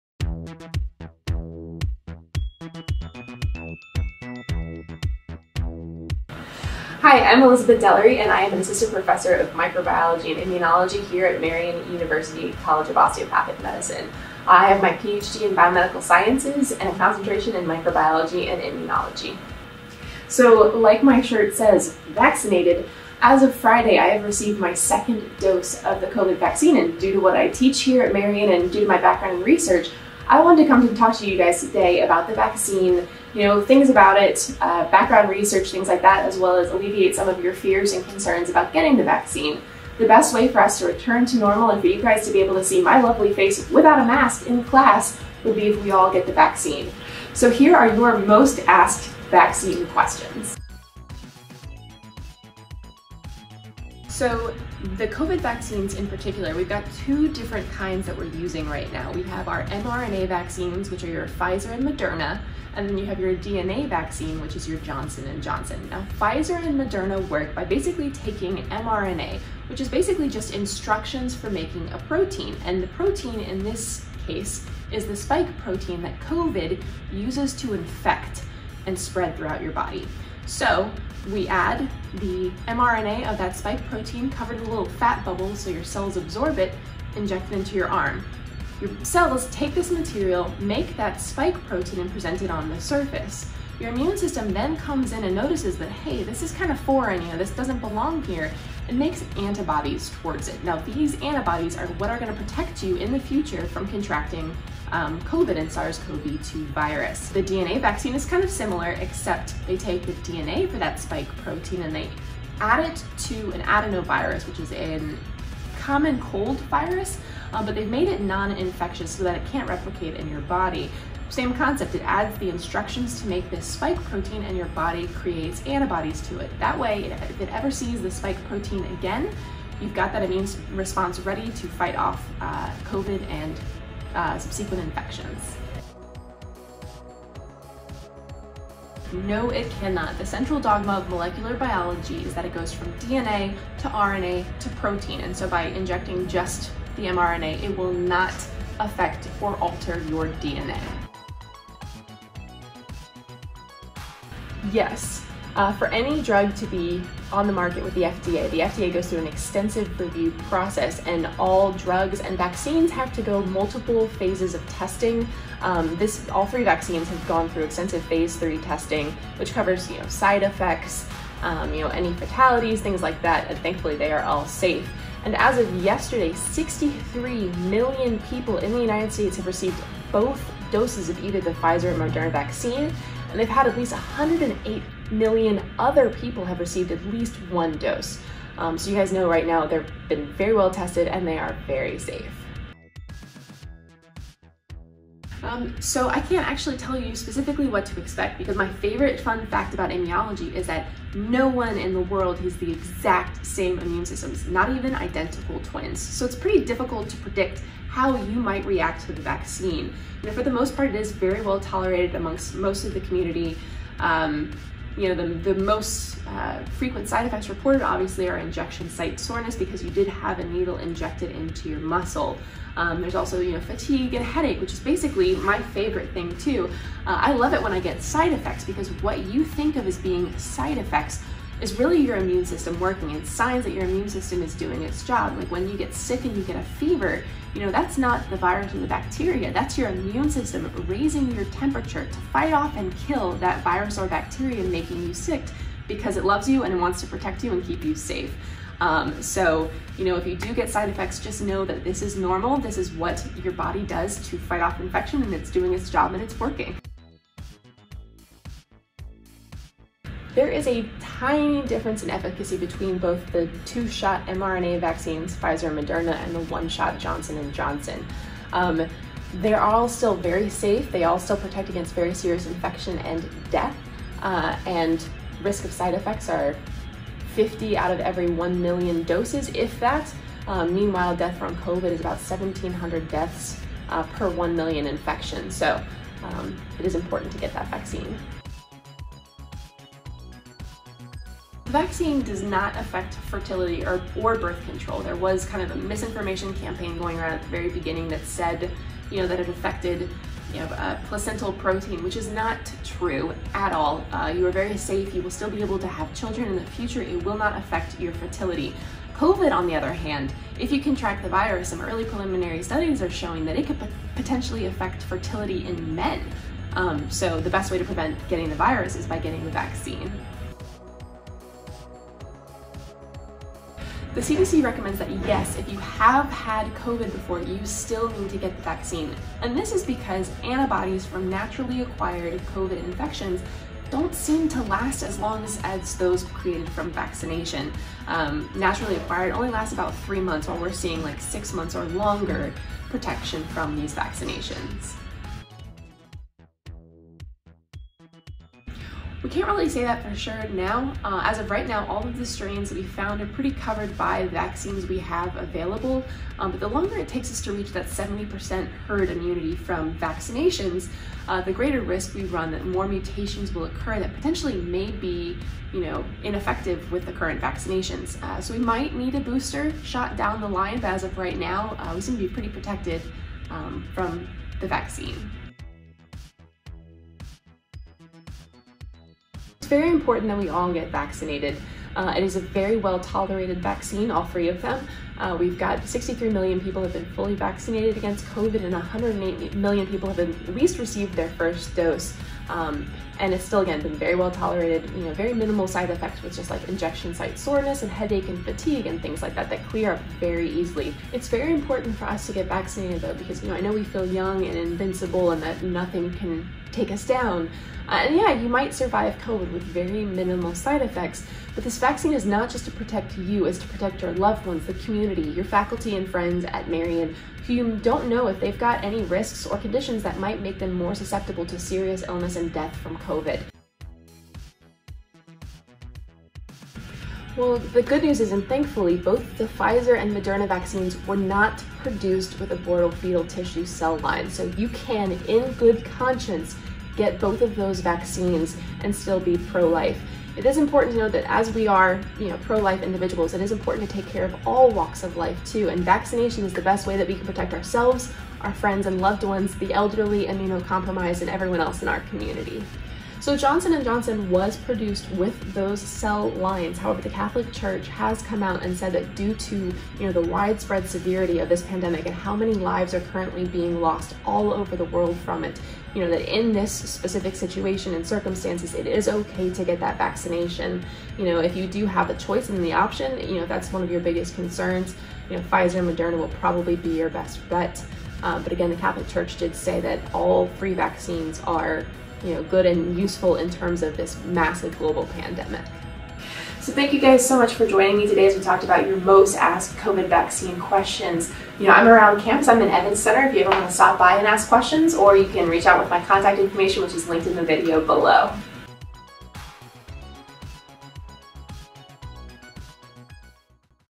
KnightCare: COVID-19 Vaccine Q&A with Dr. Delery
Elizabeth Delery, Ph.D., assistant professor of microbiology and immunology in the Marian University College of Osteopathic Medicine, answers your questions about the COVID-19 vaccine.

Questions featured in the video (with timestamps):

0:51 - How do the vaccines work?
2:16 - Can the mRNA vaccine alter my DNA?
2:31 - Have the vaccines been tested enough?
3:13 - Will I get side effects from the vaccine?
4:17 - Is there an efficacy different between the two- and one-dose vaccines? Does it Matter?
4:49 - Does the vaccine affect fertility?
5:21 - If I've already had COVID-19, do I need to get the vaccine?
5:46 - Will I need to get another vaccine later on?
6:15 - "I want to wait to get the vaccine and maybe get it later.
7:04 - "I am pro-life. Is it in good conscience to get the vaccine?"